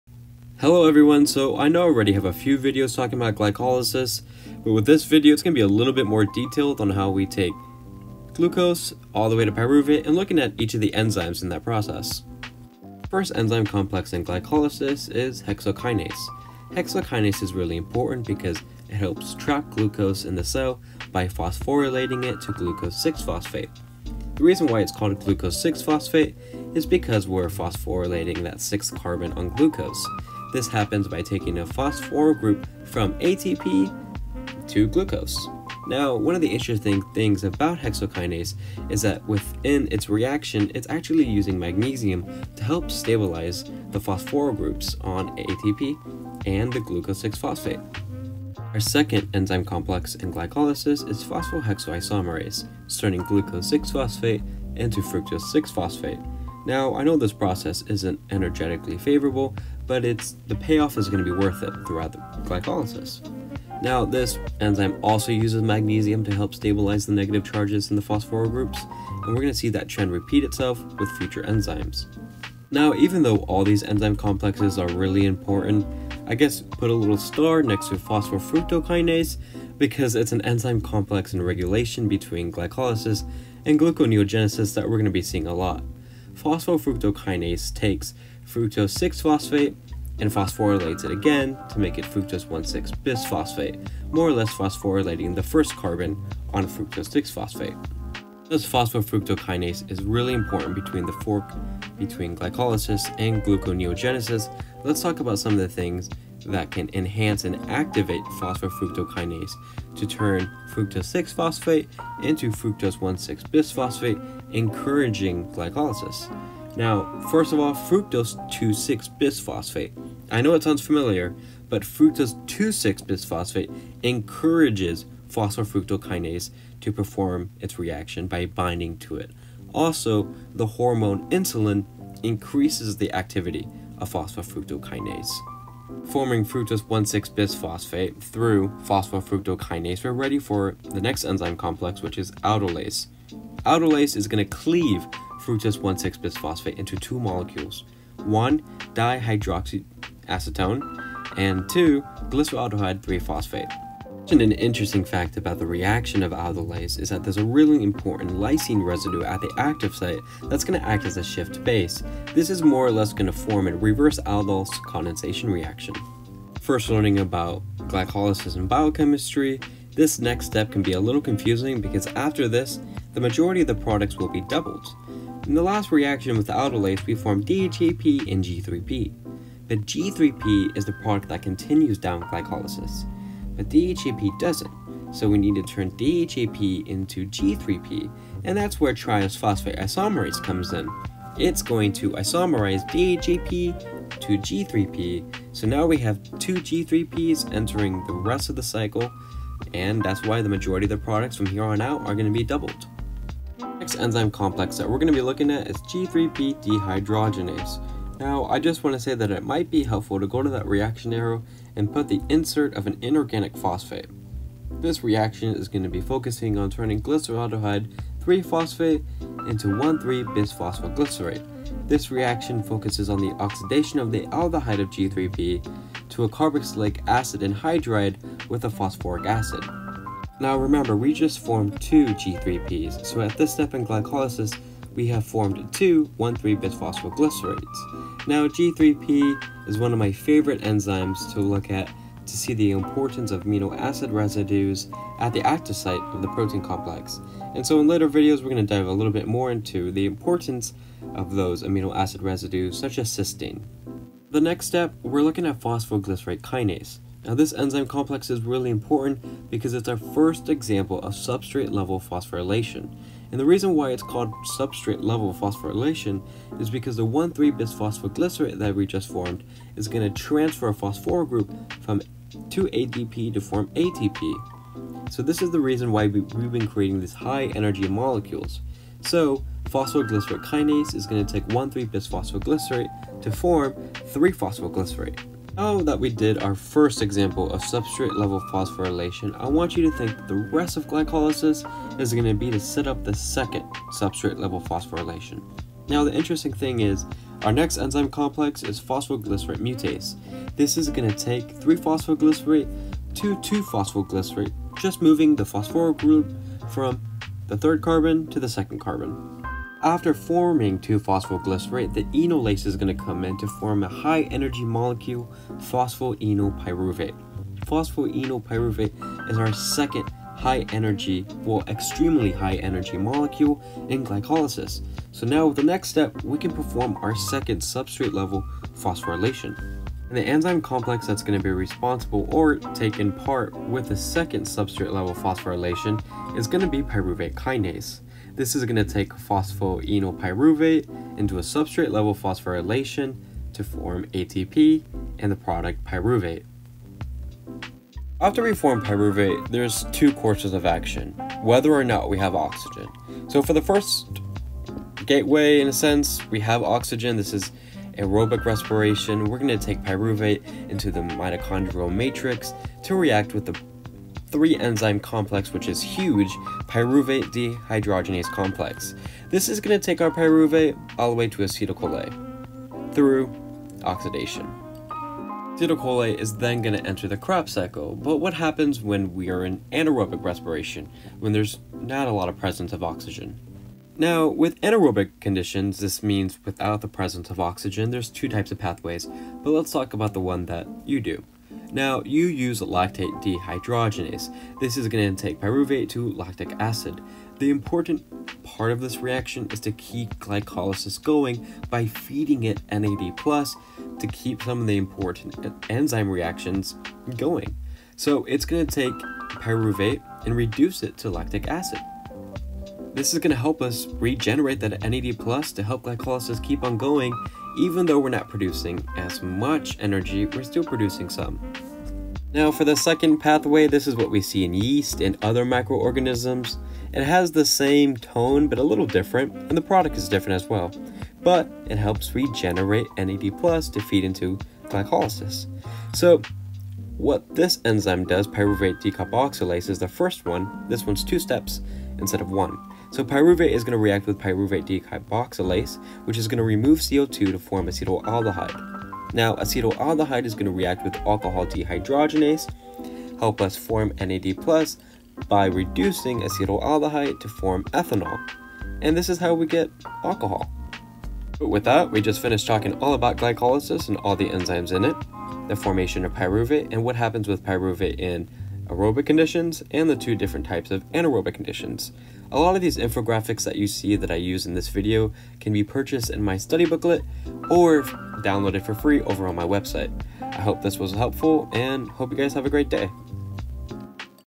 GLYCOLYSIS: Biochemical Breakdown of Glucose and All The Steps Leading to Pyruvate
Glycolysis is an important biochemical pathway that we are going to dive into and look at each of the enzymes that work to turn glucose into pyruvate.

Glycolysis is a very important reaction that starts with Hexokinase and ends with Pyruvate Kinase. Turning Glucose into Pyruvate, but what happens after? in this video, we are also going to explore what happens after we form pyruvate. The outcomes are different for both aerobic and anaerobic conditions.